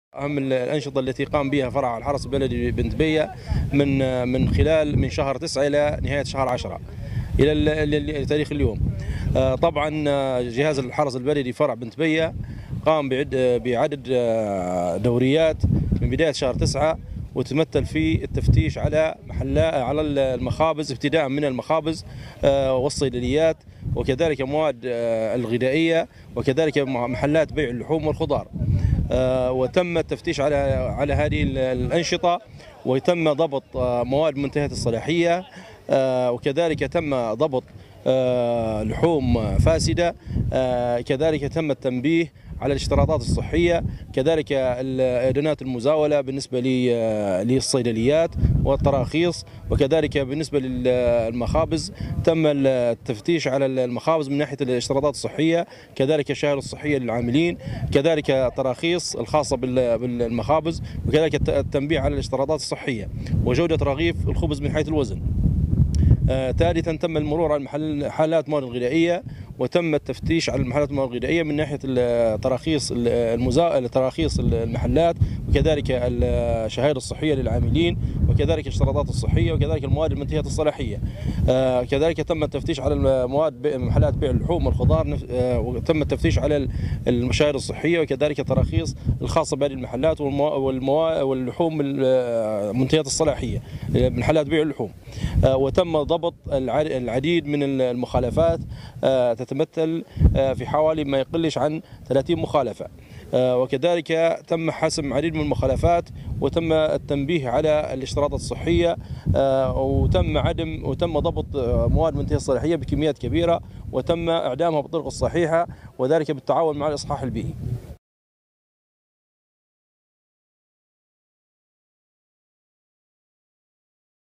(30) مخالفة رصدتها دورية الحرس البلدي ببنت بية خلال ستة أشهر LCNA
وكالة الغيمة الليبية للأخبار - بنت بية.
أفاد رئيس فرع جهاز الحرس البلدي بنت بيه "محمد إسماعيل"، أن التفتيش على صلاحية المواد المستهلكة خلال النصف الأول من هذا العام نظمت بشكل دوري على مختلف المحال التجارية والصيدليات والمخابز والمقاهي.